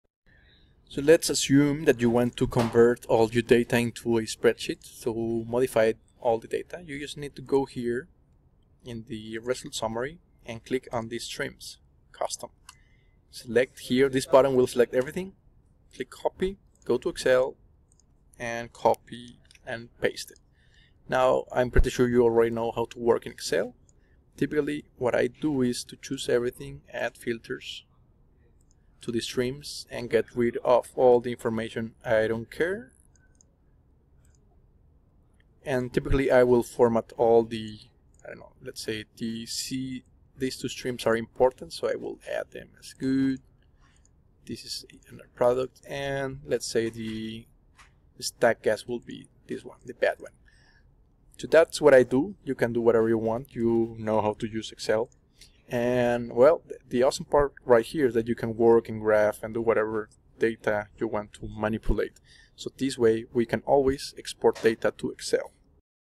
Exporting Data form Aspen Plus Simulation to Excel Spreadsheet (Lec 023)
Exporting some basic data to excel
This is an introductory course, great for students and professionals just starting their process modeling careers.

Aspen Plus from AspenTech is one of the leading process simulation software.